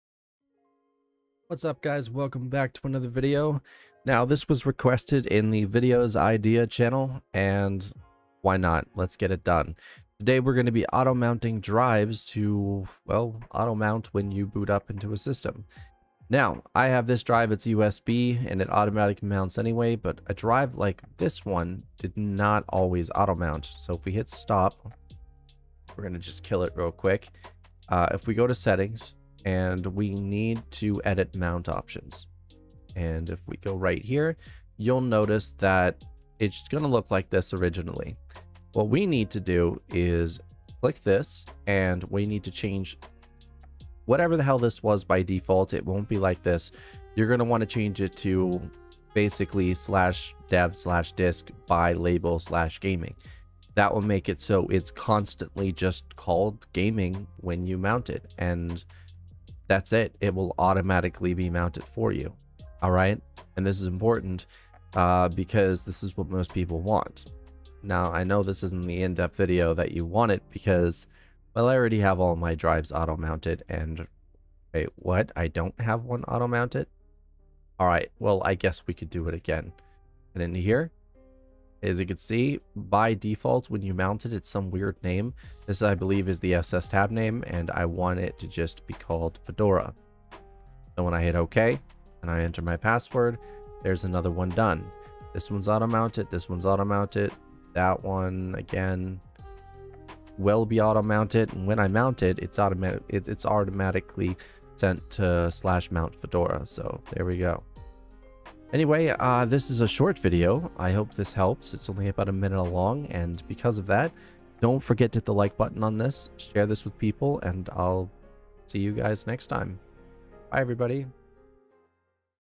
Auto Mounting Drives on Linux | Fedora | Arch | Debian | Clear Linux | Pikaos | nobara | cachyos
The tools i used is the best for the job!

Arch
sudo pacman -S gnome-disk-utility

Fedora
sudo dnf in gnome-disk-utility

Ubuntu/Debian
sudo apt install gnome-disk-utility

SPECS Below:
• OS: Fedora 40 Workstation
• OS: Nobara 40
• OS: CachyOS
• Gnome 46
• CPU: Ryzen 7 5900X (4.9GHz)
• Memory: 49 GiB DDR4 3600mhz
• Storage: 22TB
• GPU: RTX 4070